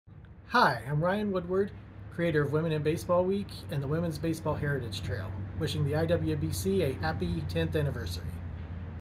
Ryan Woodward Founders Day Member Promo
IWBC Project Coordinator and Women in Baseball Week Founder Ryan Woodward wishes the IWBC a happy tenth anniversary.